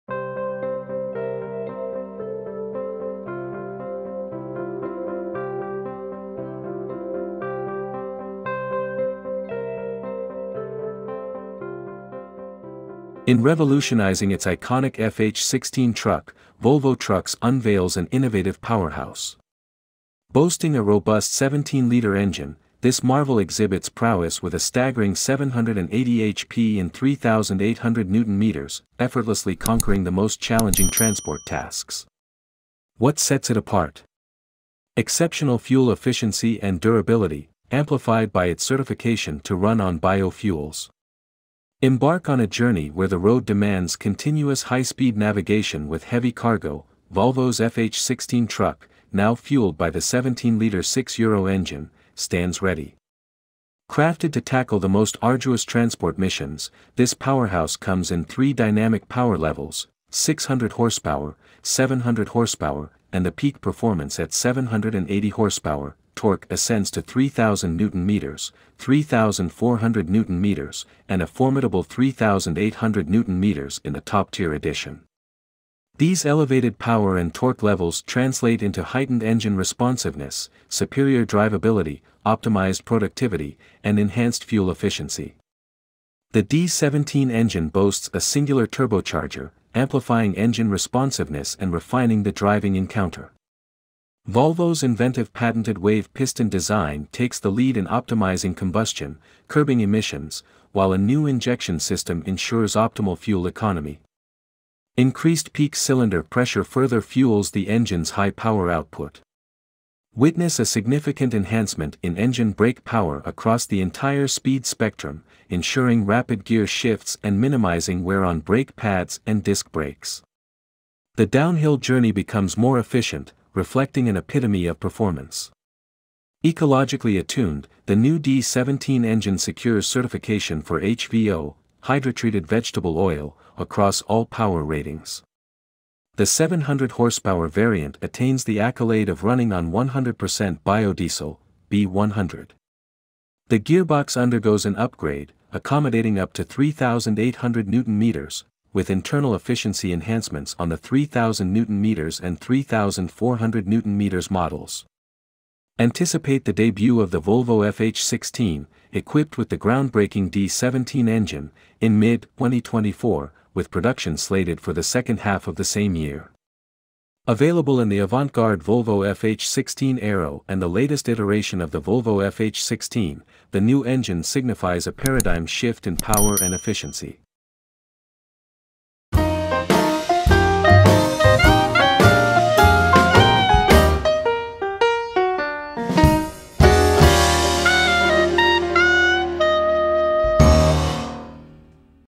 2024 Volvo FH16
Volvo Trucks introduces a groundbreaking 17-liter engine for its FH16 truck, boasting up to 780 Hp and 3800 Nm, designed to excel in demanding transport tasks with remarkable fuel efficiency and durability. The engine offers three power levels and increased torque, resulting in heightened responsiveness, superior driveability, and improved productivity. Innovations include a single turbocharger, patented wave piston design, and an efficient injection system, enhancing combustion and reducing emissions. Notably, the D17 engine is certified for eco-friendly fuels, including HVO and 100% biodiesel. The gearbox has been upgraded for optimal performance. Sales for the Volvo FH16 with the new engine commence in mid-2024, with production set for the second half of the same year, marking a significant leap in power and efficiency for the FH16 series.